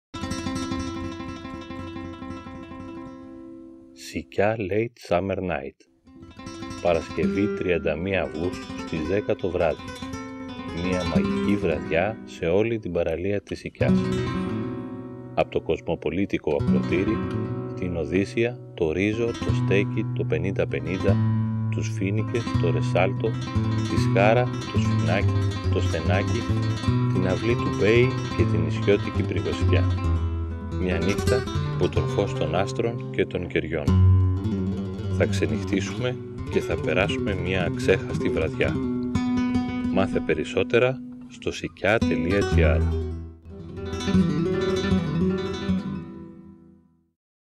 Sykia Late Summer Night
Μία μαγική βραδιά στην παραλία της Συκιάς.

Ζωντανή μουσική και
ένα φανταστικό σκηνικό,
στο κύμα της θάλασσας,
σε όλη την παραλία της Συκιάς.

Από το κοσμοπολίτικο Ακρωτήρι,
στην Οδύσσεια, τον RIZO, το Στέκι, το 50-50, τους Φοίνικες, το Resalto, τη Σχάρα, το Σφηνάκι, το Στενάκι, την Αυλή του Μπέη,
την νησιώτικη Πυργοσυκιά.

Με διαφορετική ζωντανή μουσική και
χαλαρές μελωδίες,
όπως ταιριάζει
στην τελευταία αυτή νύχτα του Αυγούστου.
μια νύχτα υπό το φως των άστρων και των κεριών.

Θα ξενυχτήσουμε και θα περάσουμε μία αξέχαστη βραδιά.